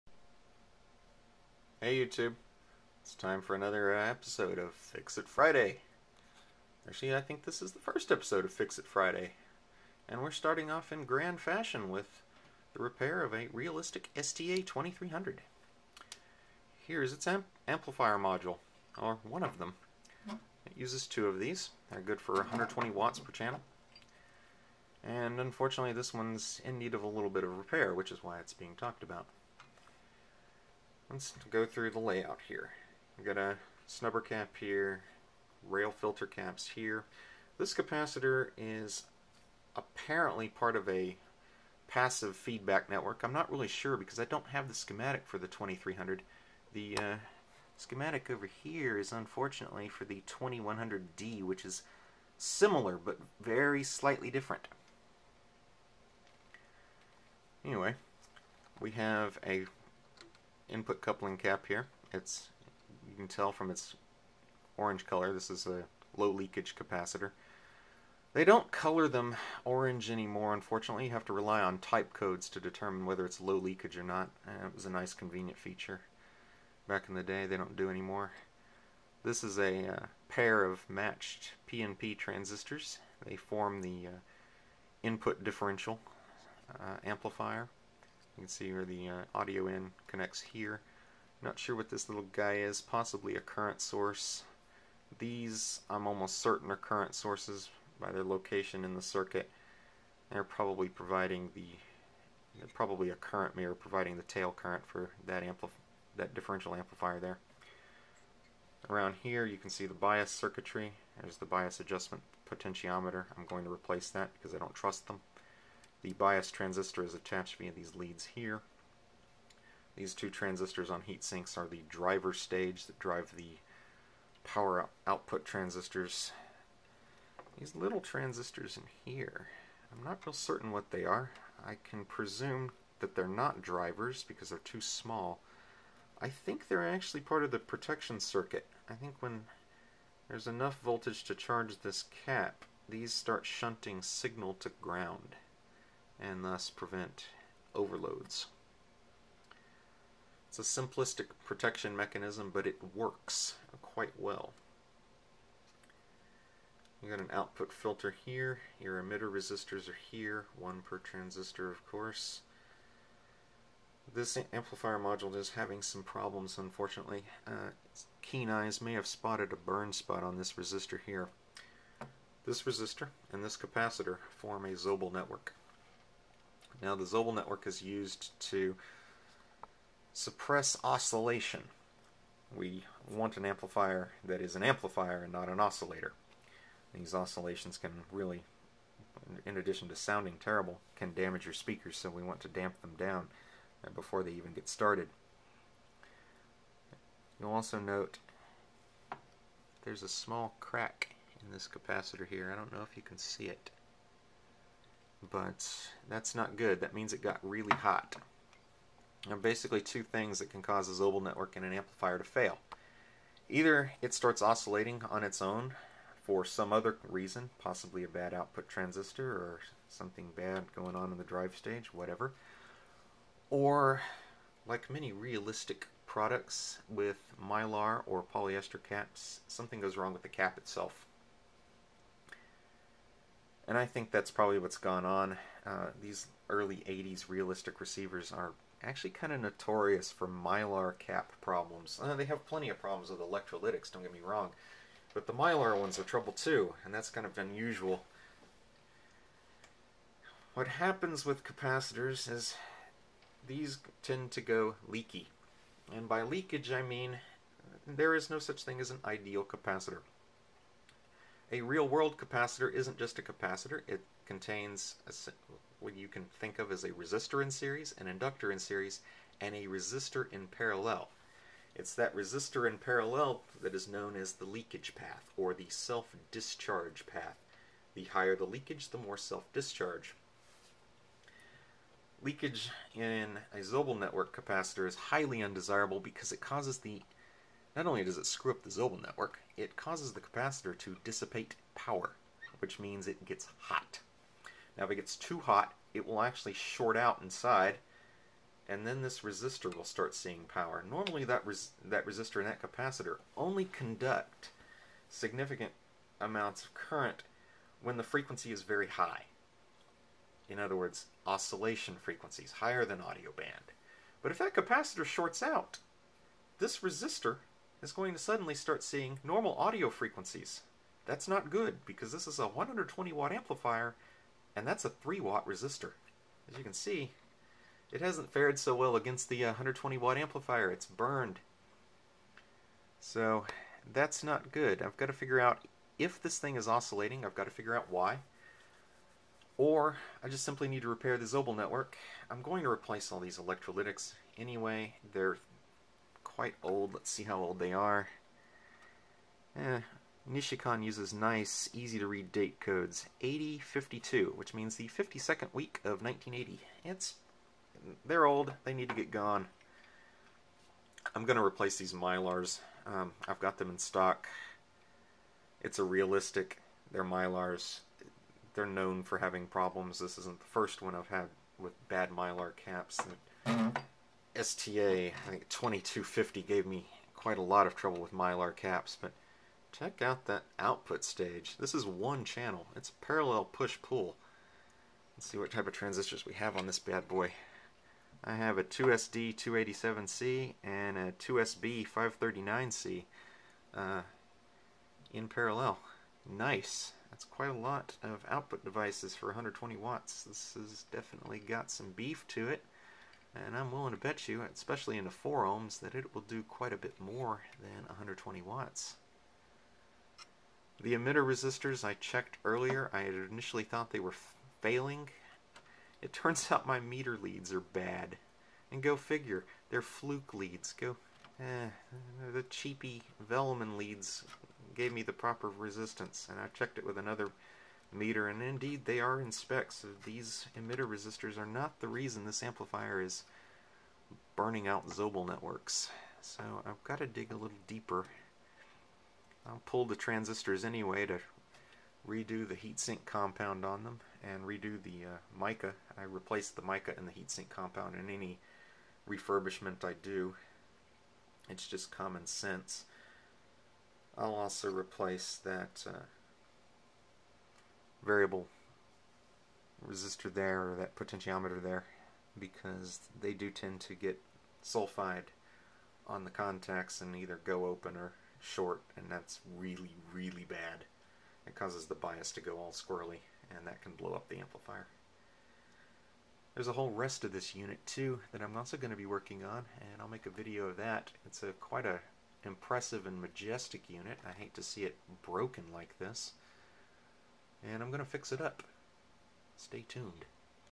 Fix It Friday: Realistic STA-2300 Power Amplifier Modules
A big Realistic STA-2300 receiver finds its way to Andrew's Electronics (a.k.a The Mad Scientist's Lair). In this video I walk you through the various parts of the STA-2300's amplifier modules and discuss the problems mine are having. This amp has either become unstable or this is yet another case of capacitor plague...this time with polyester/Mylar capacitors instead of electrolytic caps.

Please accept my apologies for the illegible text on the PCB...the Flip HD doesn't like macro. (Buy my refurbished stereos or send me your broken one for repair so I, a poor EE student, can get a better camera!)

Trivia: Contrary to popular myth fueled by the striking visual and performance similarities with the Pioneer SX-1080, the STA 2100 and 2100D were NOT made by Pioneer. They were custom made for Radio Shack by Foster Electronics and designed to compete directly with the SX-1080 which they do very well. Taking the lids off of both reveals substantial differences in layout between the Pioneer and the Foster made Realistic units.

The later 2300 is essentially a 2100D without the Dolby FM circuit and LED meters instead of mechanical movements. Like the 2100 and 2100D, the 2300 was made by Foster. When faced with a question of a 2100D or 2300, its all down to taste: Do you prefer the classic look of mechanical meters or the 80s retro chic look of lots of LEDs? Both pack the same punch and both are wonderful units.